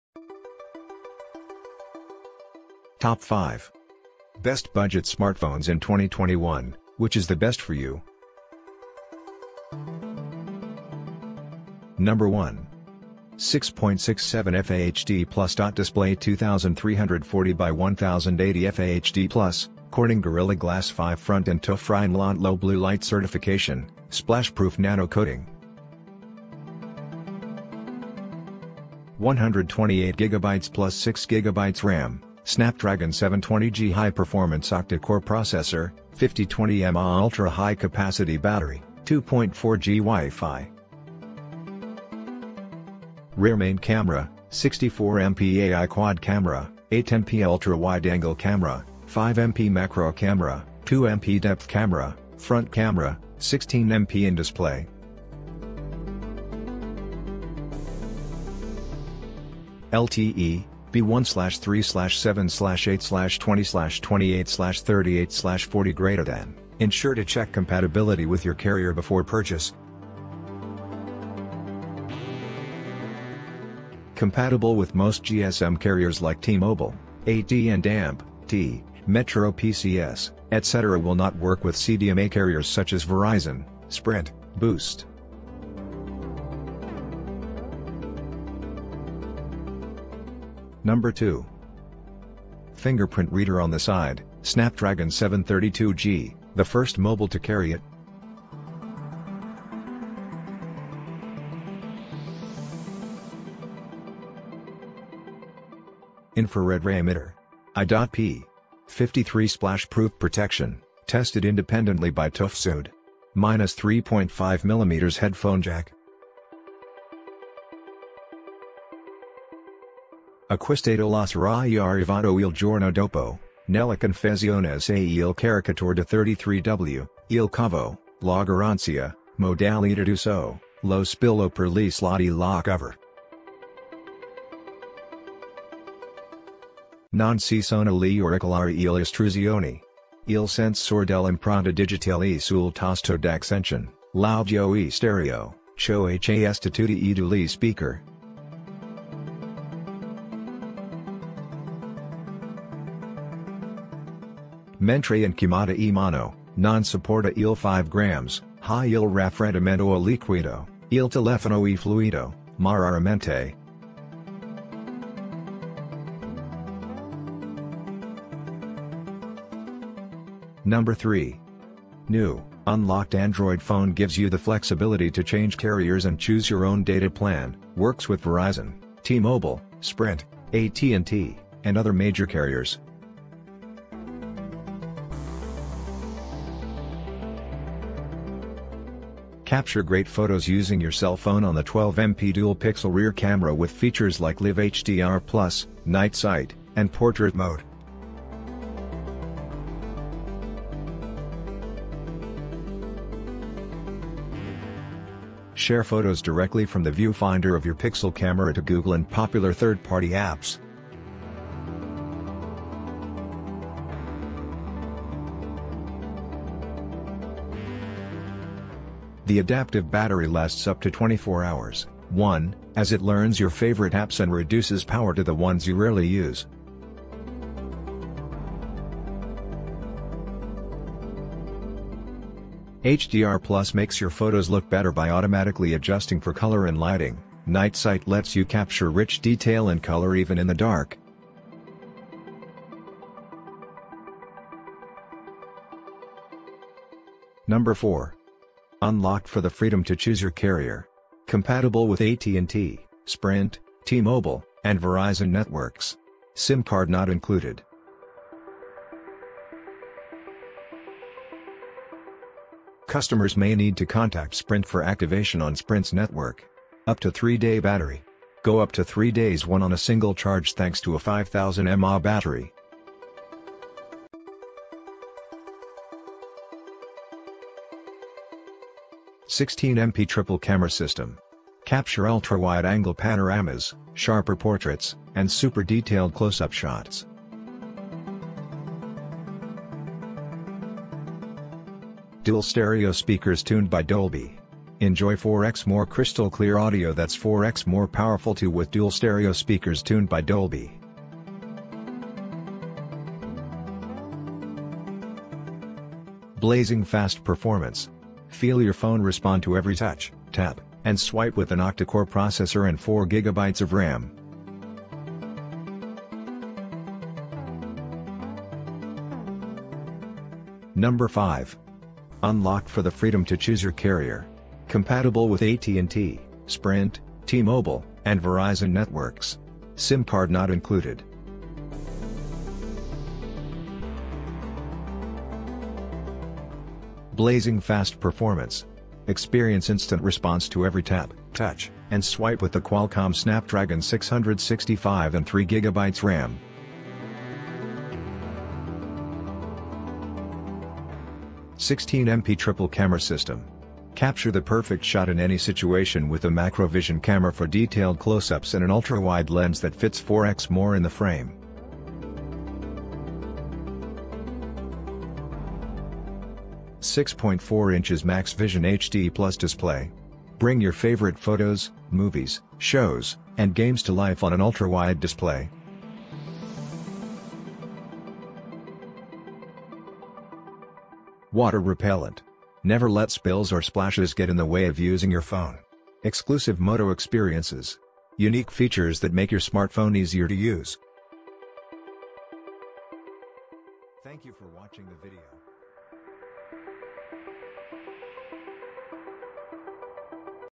Best Budget Smartphones in 2021 - Which Is The Best For YOU
► Links to the best budget smartphones we listed in this video:

► 5. Moto G Fast
► 4. Moto G Power
► 3. Google Pixel 4a
► 2. Xiaomi Poco X3
► 1. Xiaomi Redmi Note 9 Pro